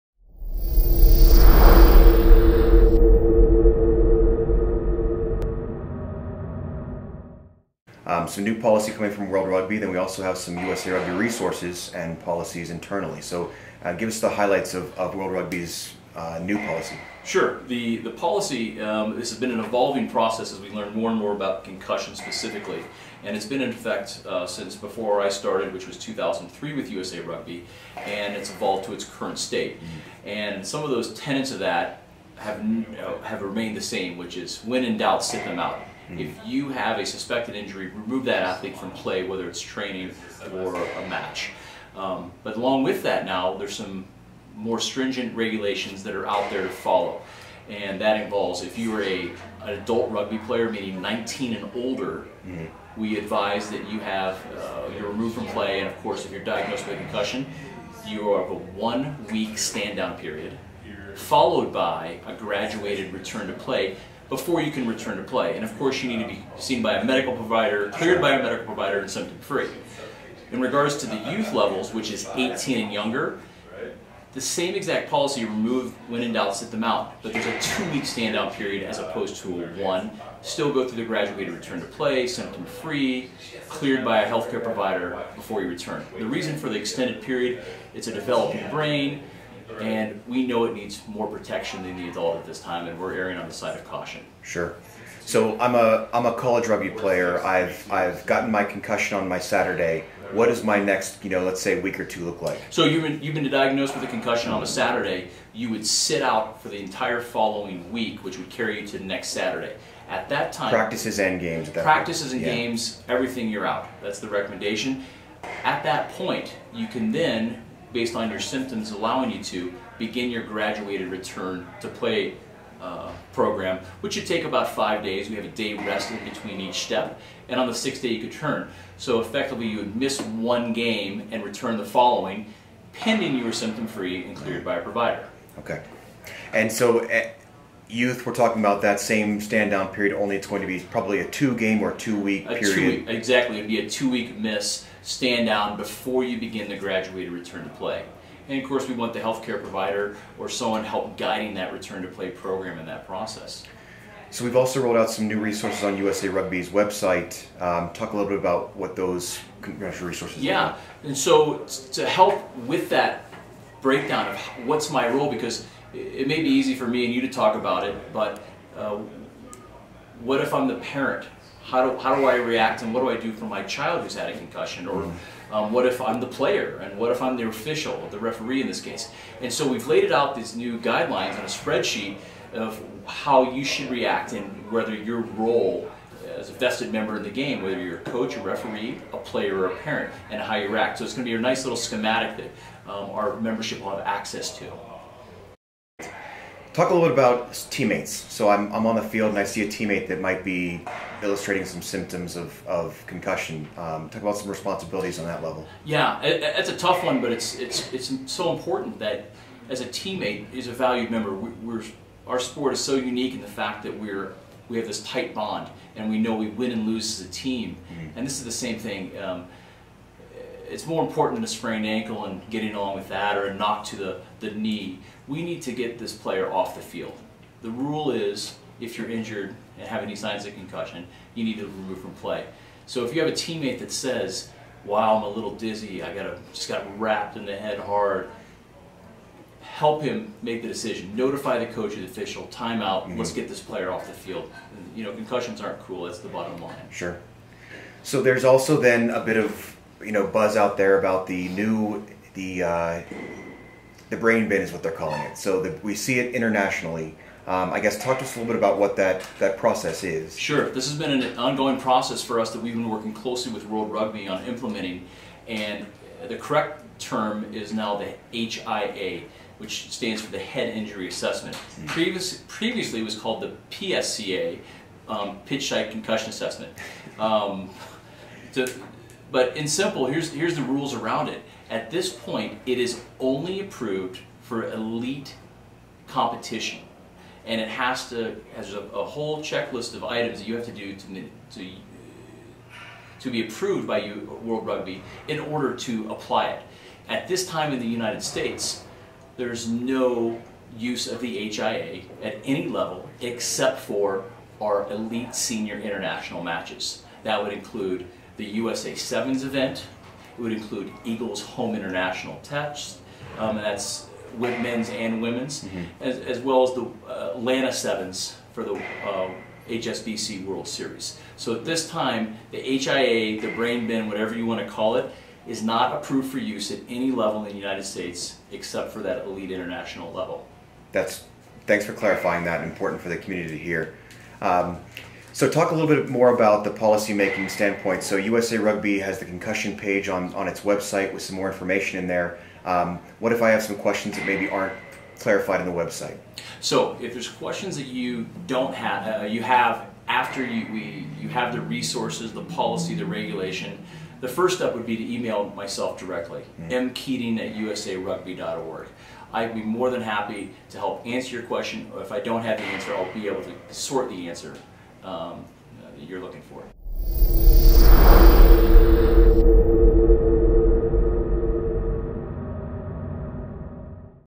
USA Rugby's Concussion Policy with Michael Keating
Learn more about USA Rugby's concussion policy through Michael Keating, Director of Medical Services.